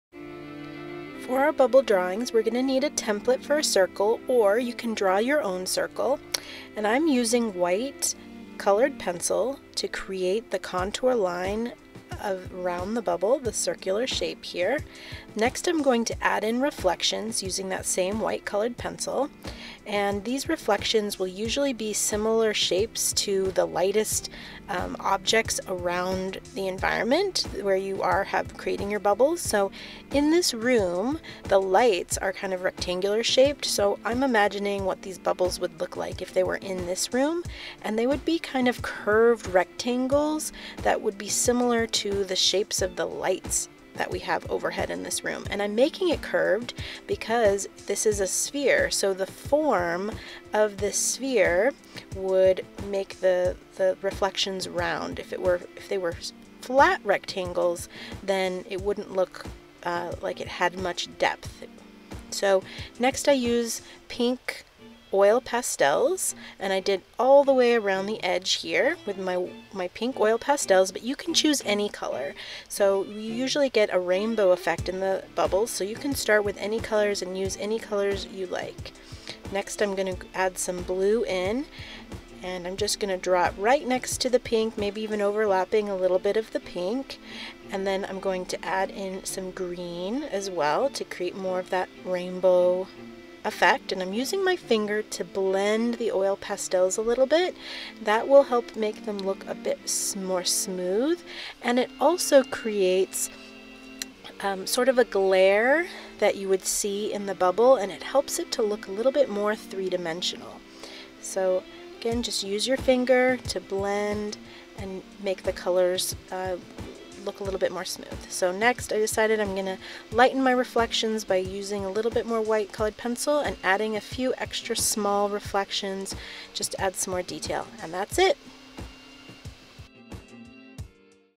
How to Draw a Bubble
Simple bubble drawing with white colored pencil and oil pastel.